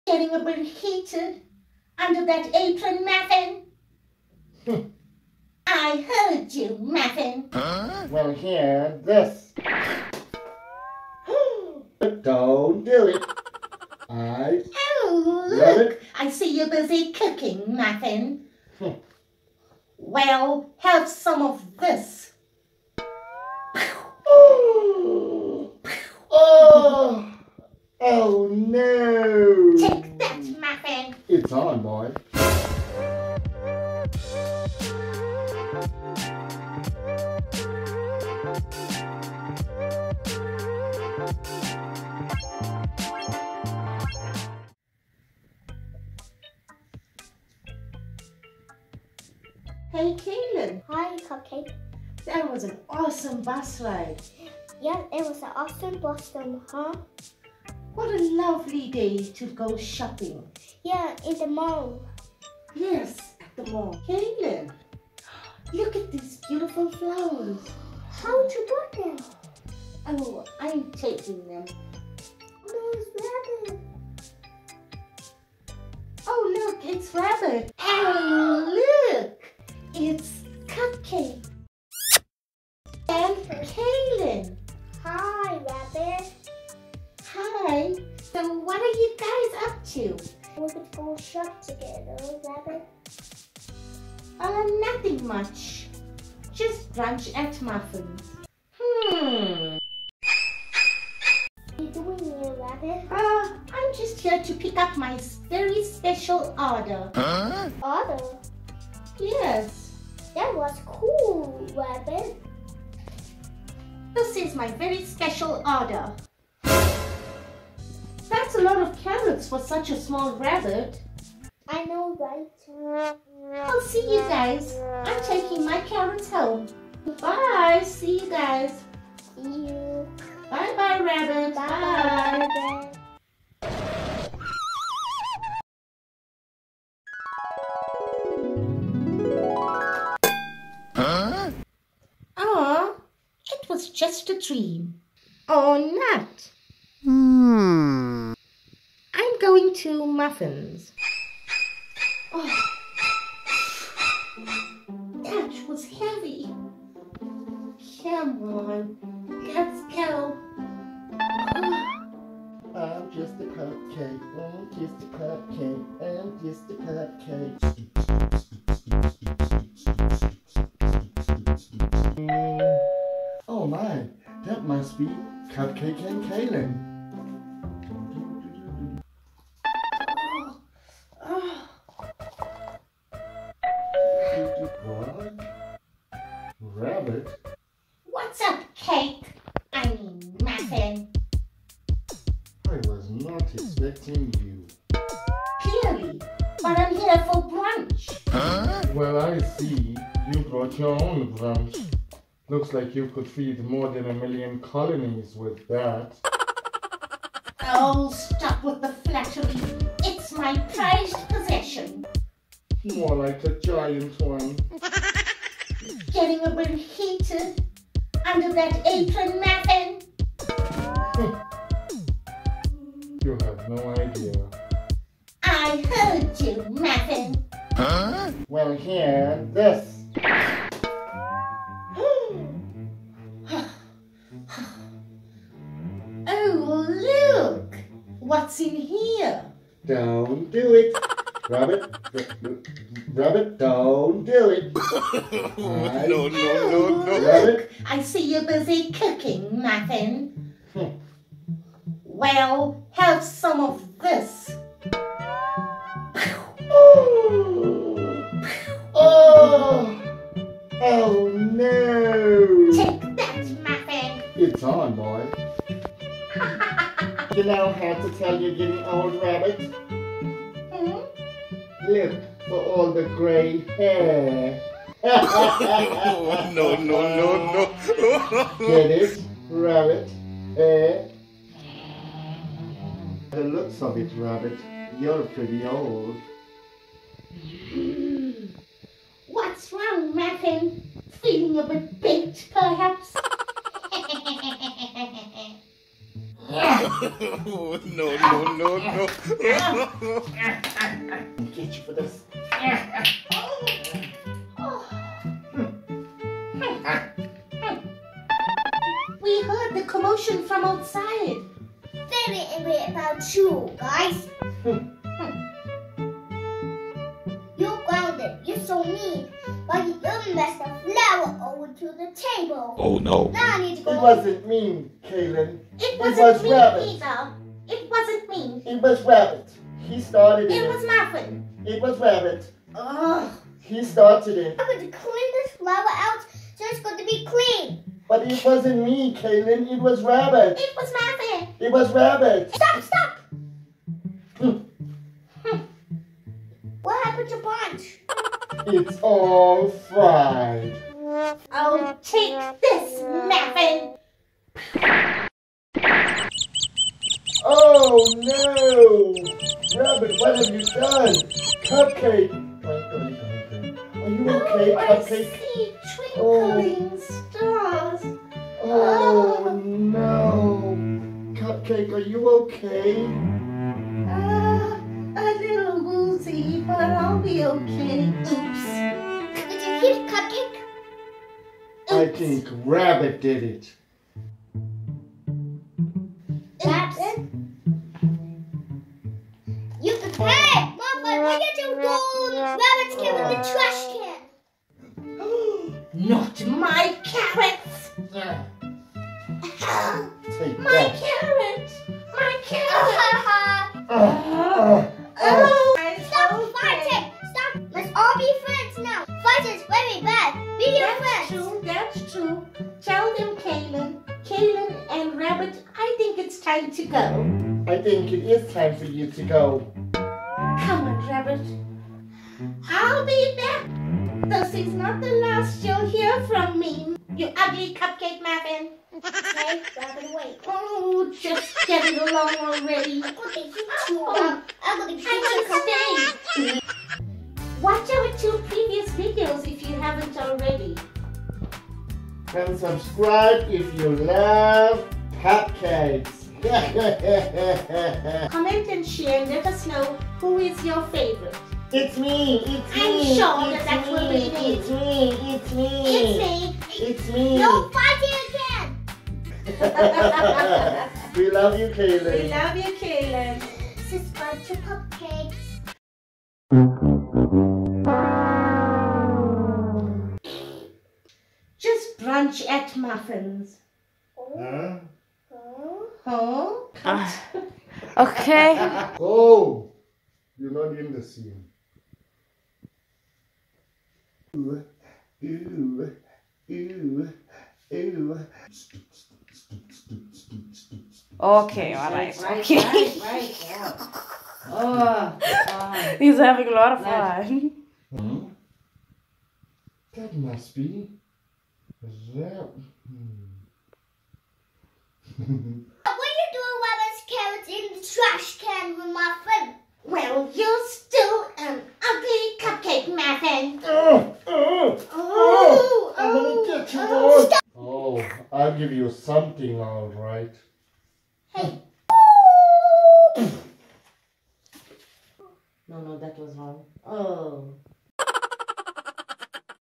Puppet Fun: An Uninvited Guest😠😤😱
Muffin is cooking for Kaylen and Cupcake. But an unexpected visitor shows up and ruins things...

Hi, I'm Kaylen. Come play with me, dance with me and go crazy with me!